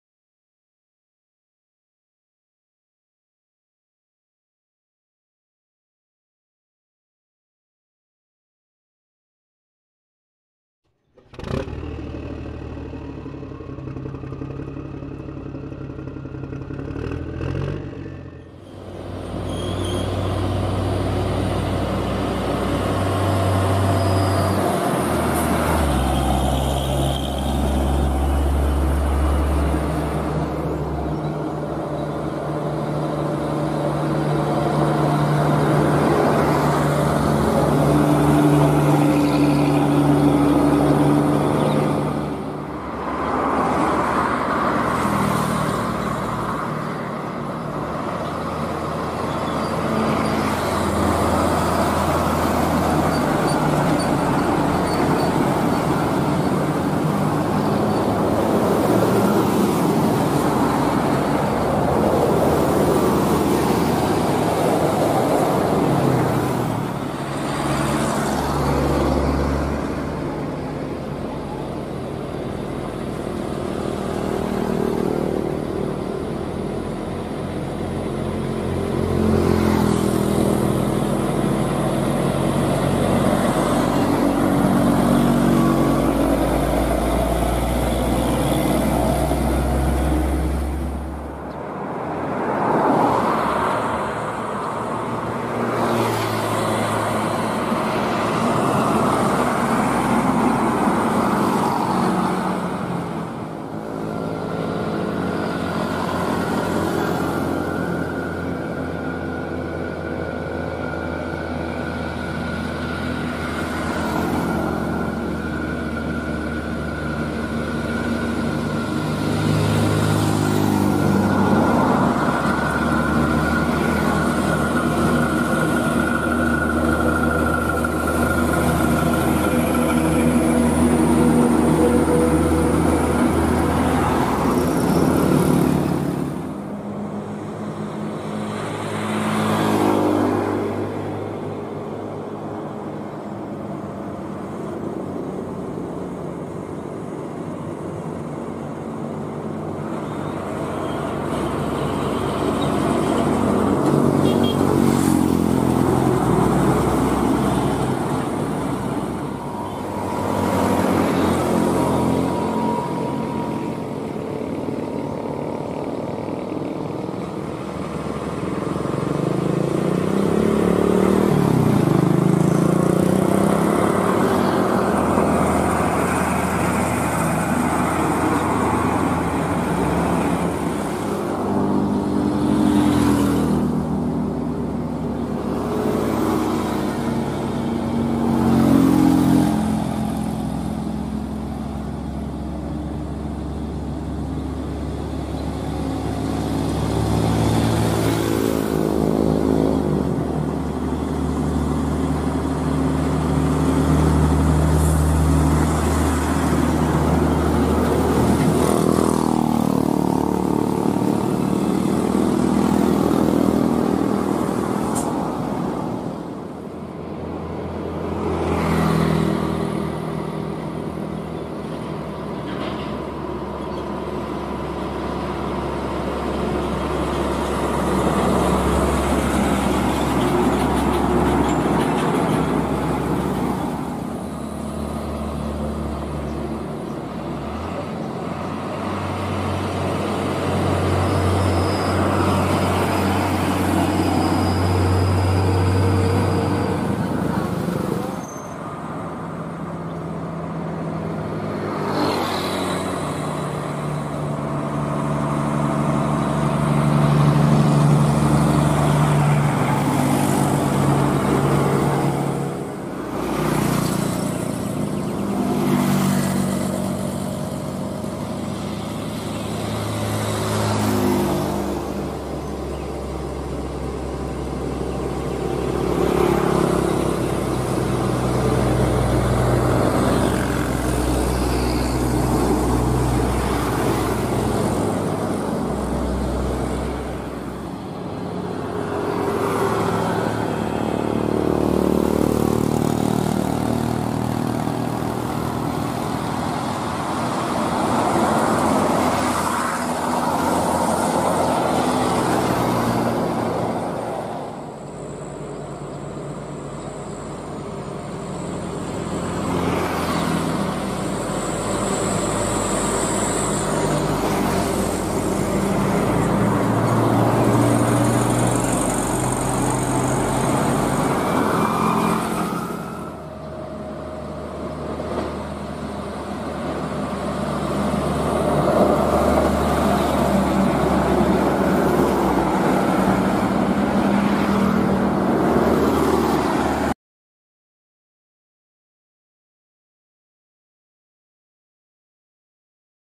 UDARA YANG MASIH SEJUK UNTUK MASUK INTAKE MANIFOL TRUK // PANTURA DENDELS
pagi yang sejuk , sebagian truk logistik sudah beroperasi dijalur Pantura Lamongan
Salam 1 Aspal 🙏🇲🇨